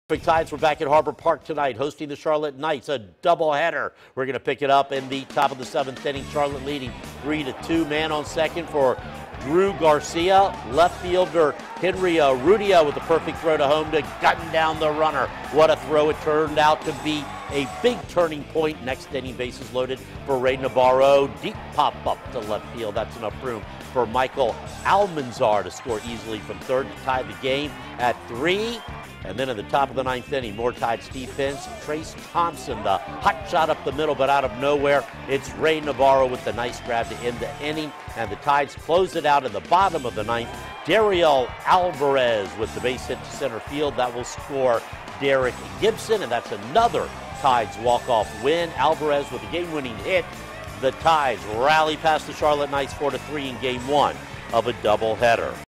Dariel Alvarez walk-off hit leads Tides past Charlotte 4-3
Dariel Alvarez helped lead the Norfolk Tides to a 4-3 win over the Charlotte Knights.